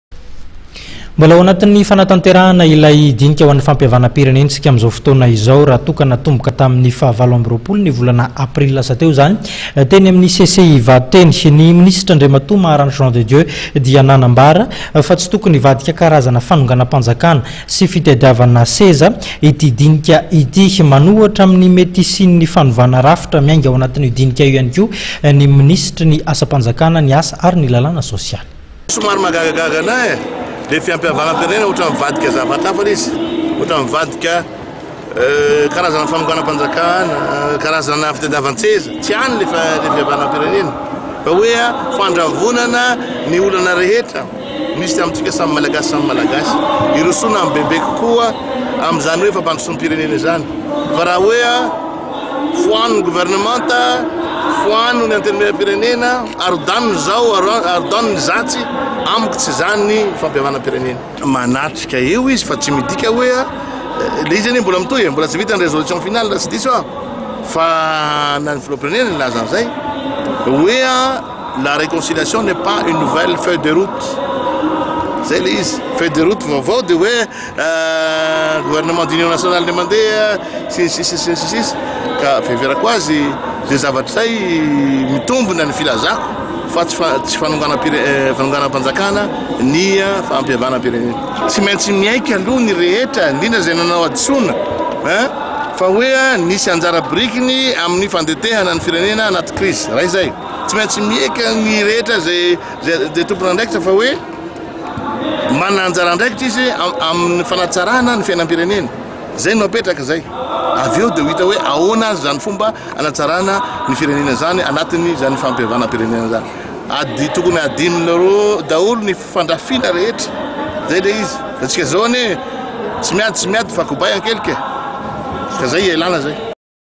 8 MAHARANTE
N a n o m b o k a   n y   2 8   A p r i l y   l a s a   t e o     n o   n o t a n t e r a h a n a   t e n y   a m i n ' n y   C C I   I v a t o   n y   f i h a o n a m b e   h o   a n ' n y   f a m p i h a v a n a m - p i r e n e n a .   N a m b a r a n y   n y   m i n i s t r a   M a h a r a n t e   J e a n   D e   D i e u   f a     t s y   t o k o n y   h i v a d i k a   k a r a z a n a   f a n o n g a m - p a n j a k a n a   s y   f i t a d i a v a n - t s e z a   n y   d i n i k a .